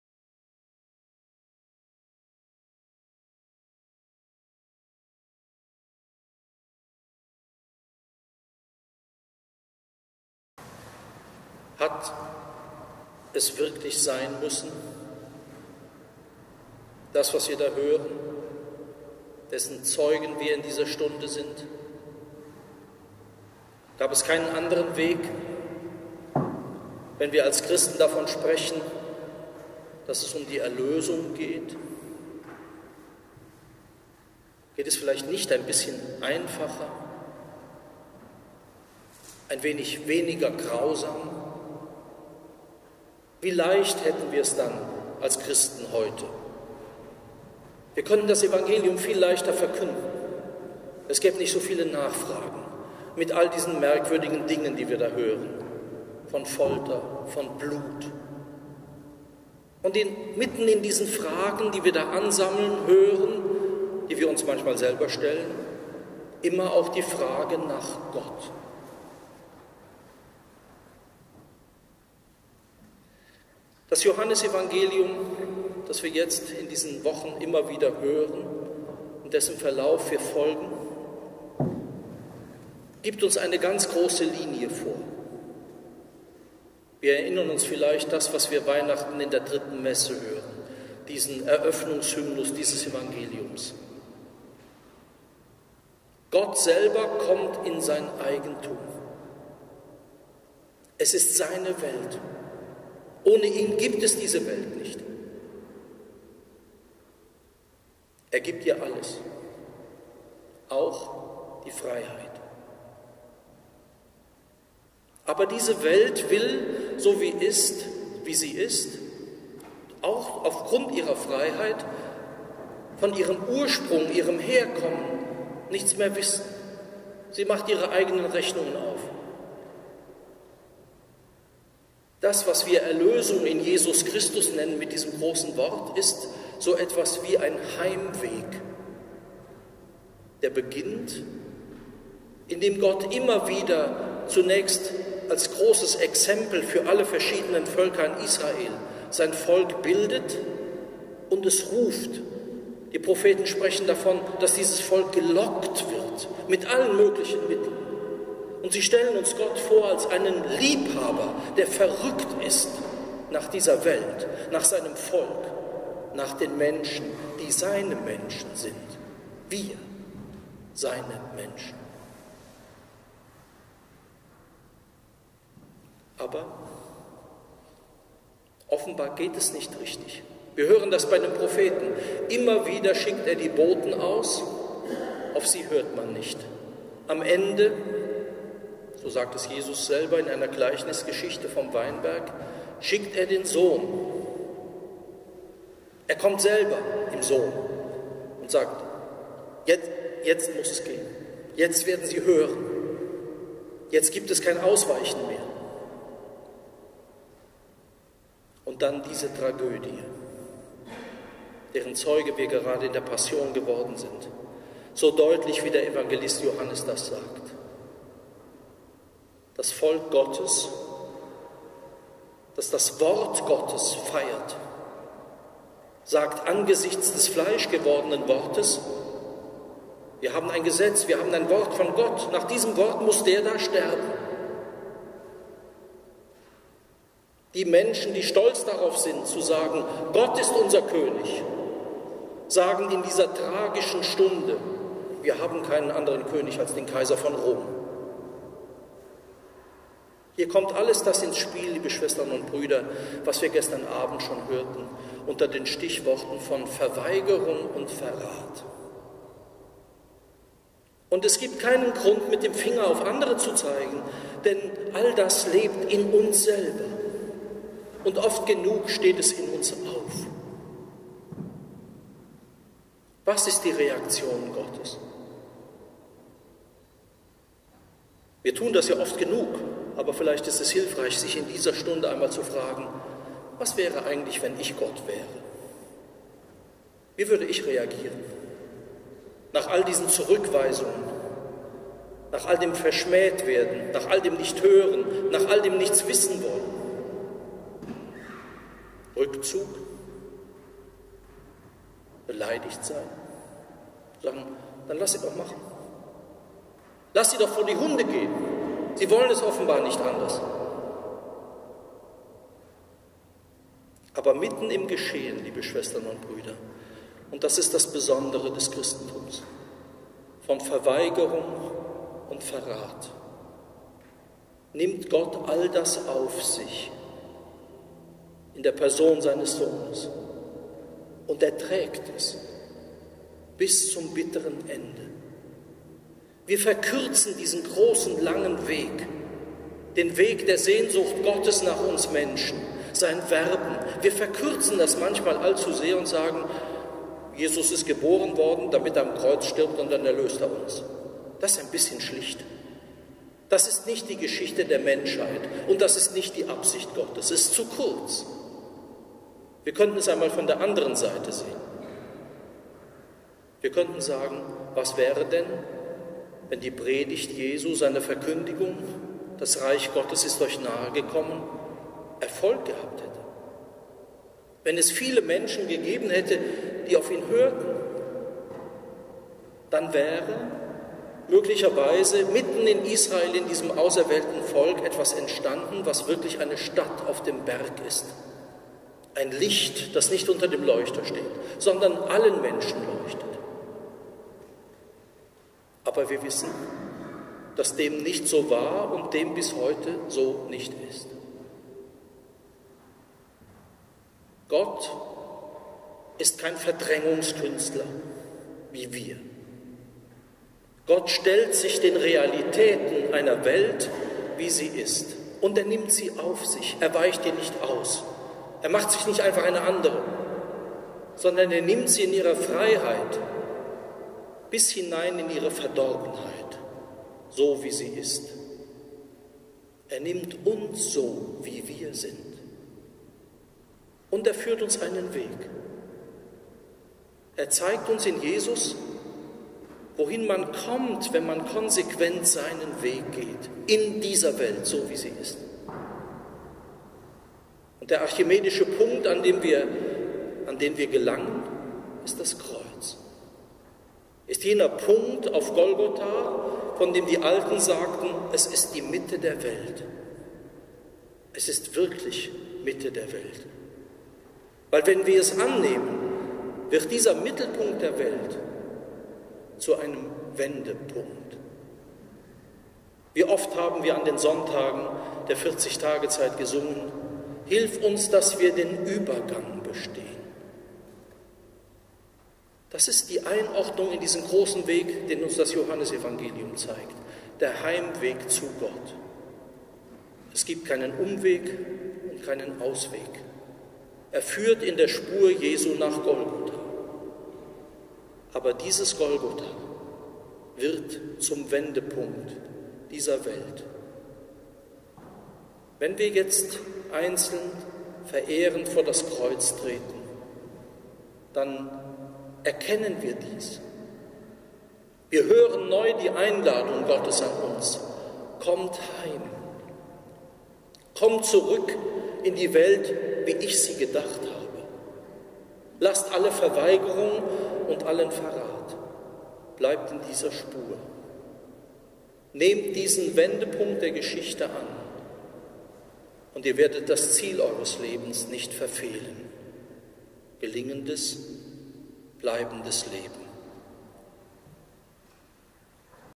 Hat es wirklich sein müssen?
Predigt von Rektor Dr.Johannes J.Kreier am Karfreitag 2018 im Kloster am Rande der Stadt/ SB .